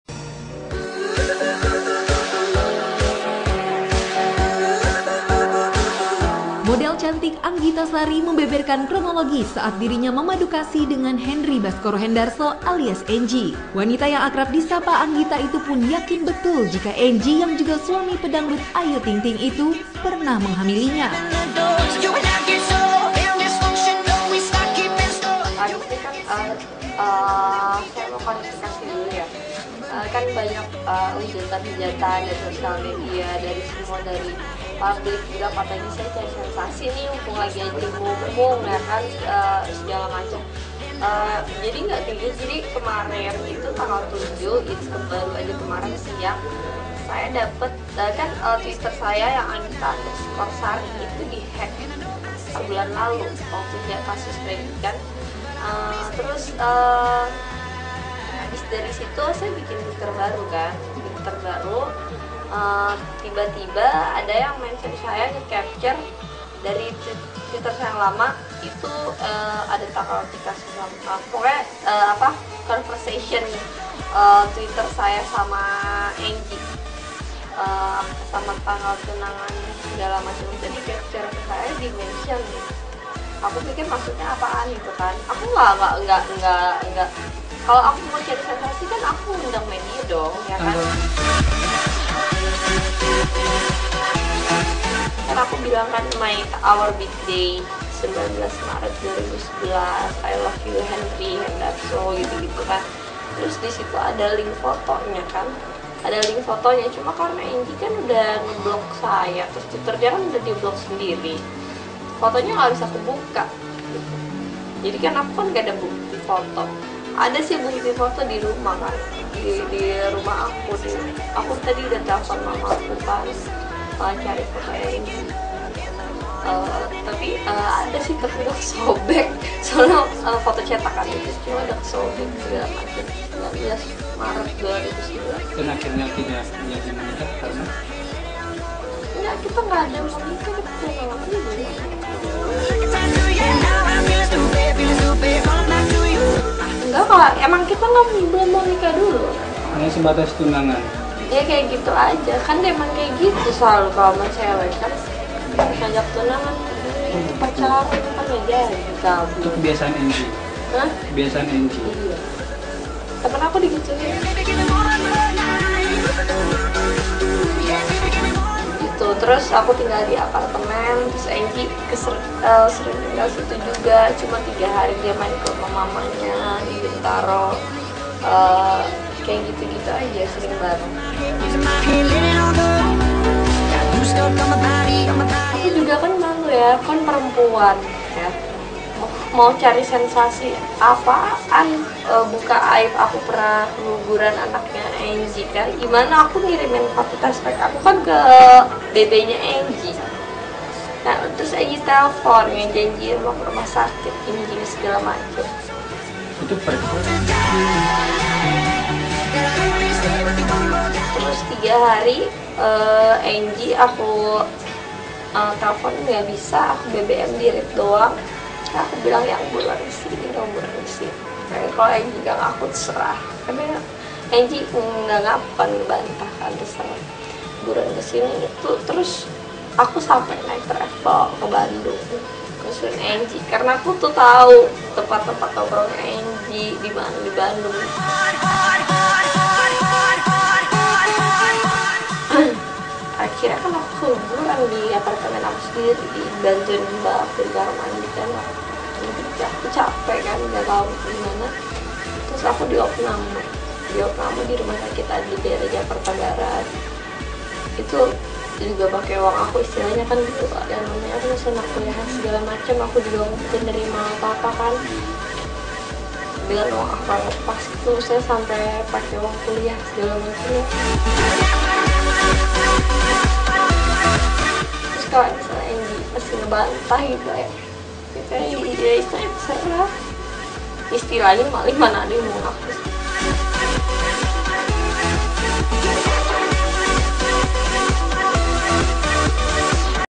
Anggita Sari Mengaku Pernah dihamili Enji dan Keguguran!
Anggita sari menceritakan bagaimana kisah cinta antara dirinya dengan enji yang pernah terjalin. Dan sempar mengandung bayi enji namun mengalami keguguran dikemudian hari.